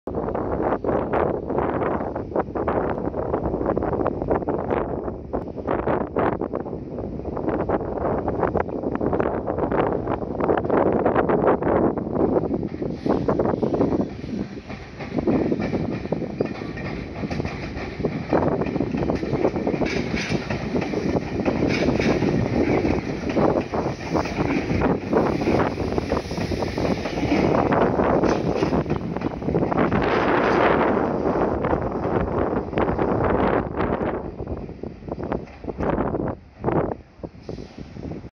●20190814 5700系 到着 ＠新安城
撮影路線：名鉄 名古屋本線
撮影駅：新安城
行先：普通 犬山
車両：5700系
両数：4両
撮影機材：【スマホ】SONY Xperia XZ2 Premium
MH：ーー
備考欄：